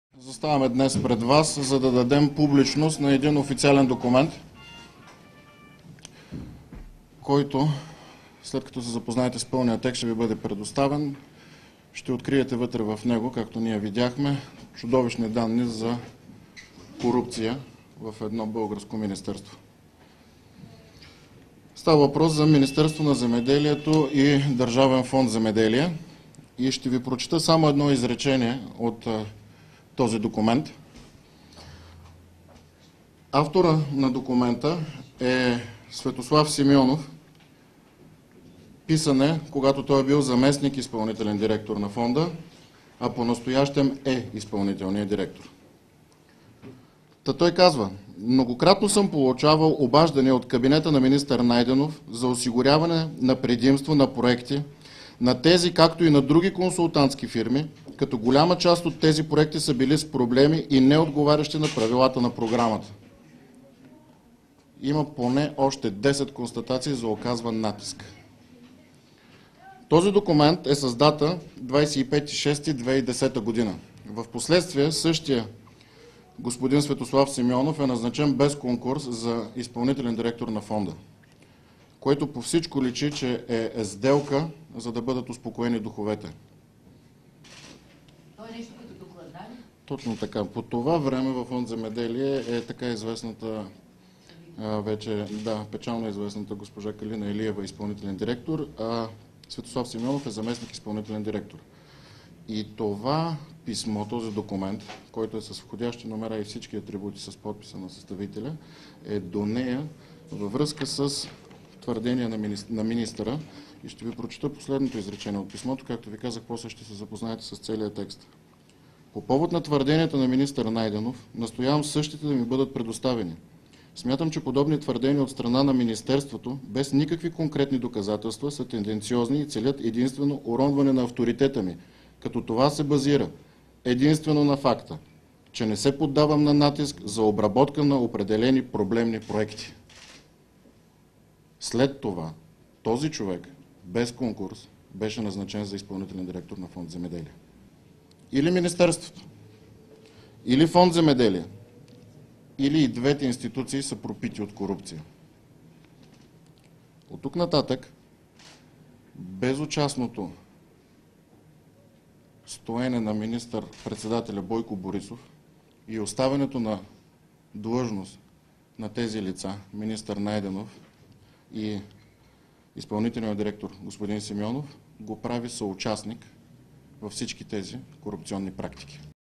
СДС с шокиращи разкрития за Мирослав Найденов
СДС предостави шокиращ документ за корупционни практики в Министерството на земеделието и храните и Държавен фонд "Земеделие" (ДФЗ).

Сигналът е бил изпратен до Парламентарната комисия за борба с корупцията. Тя от своя страна е сезирала прокуратурата, но отговор до ден - днешен няма. Това заяви Борис Марков, председател на предизборния щаб на СДС. В документа, който бе показан, има писмени обяснения от Светослав Симеонов, заместник - изпълнителен директор на фонда тогава и датата на документа - 25 юни 2010 г. Обясненията са адресирани до директора на фонда тогава Калина Илиева.